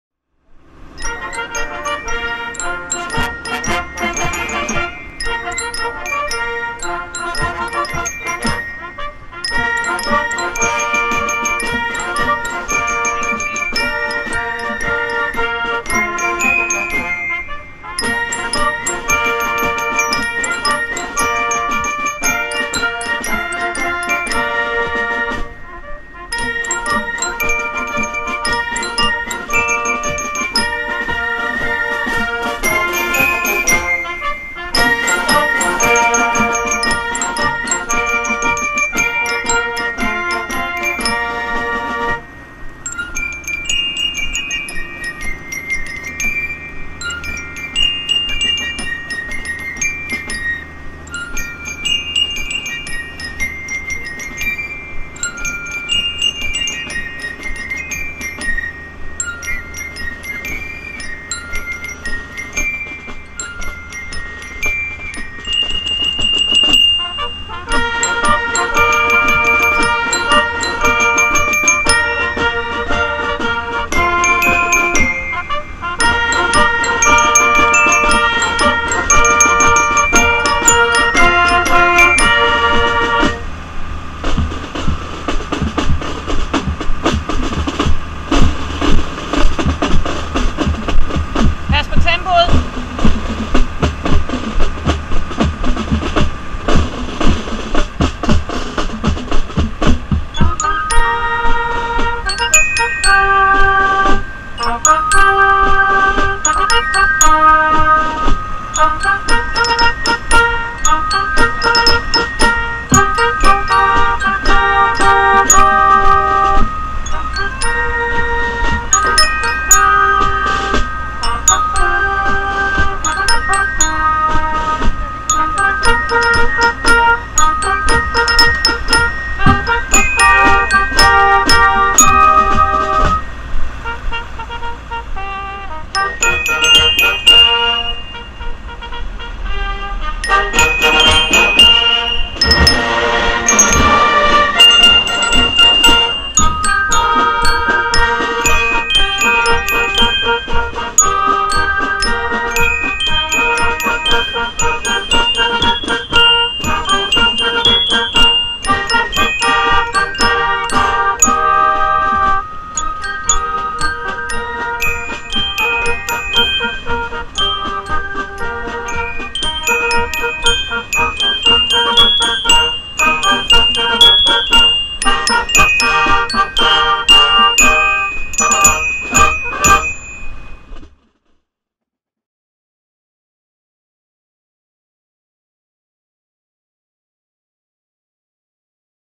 Ministævne 2016
Her er en video af de forskellige ting vi lavede under ministævnet 2016, som blev afholdt af Stenløse Tambourkorps